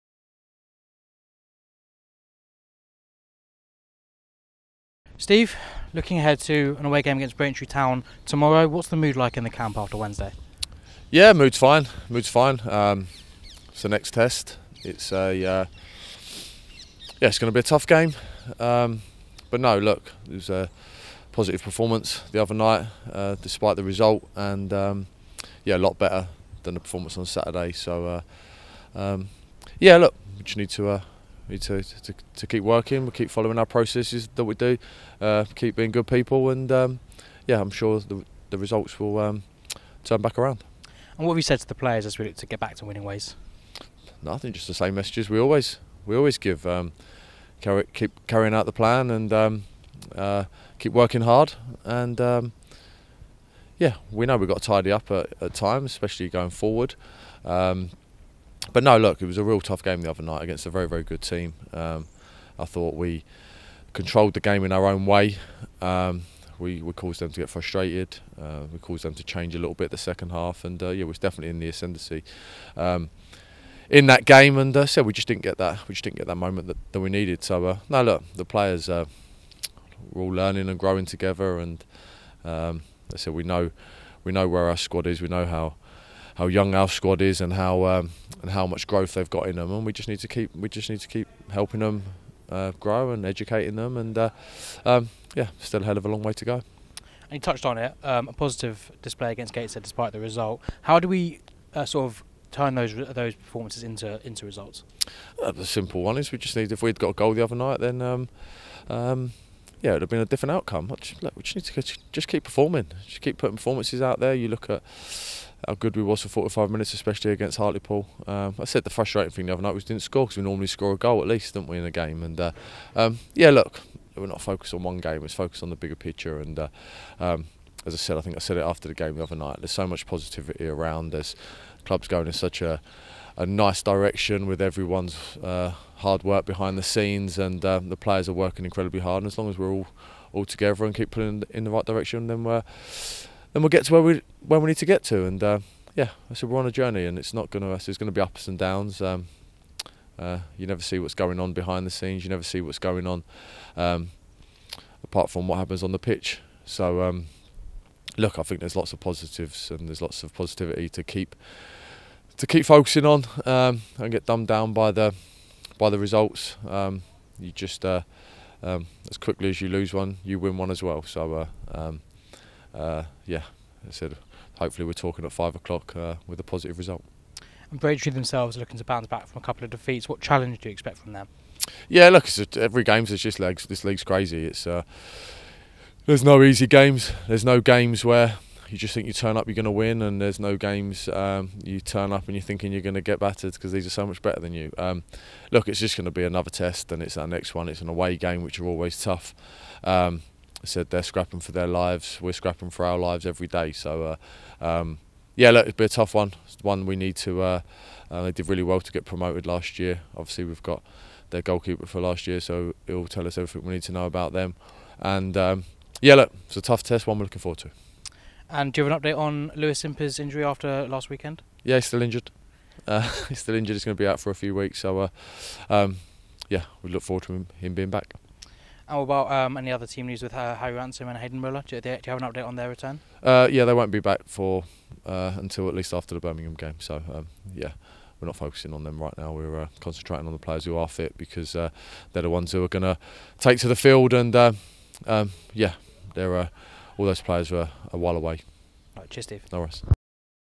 PRE MATCH Steve Morison previews away trip to Braintree 25/10/24
Photography - Paul Loughlin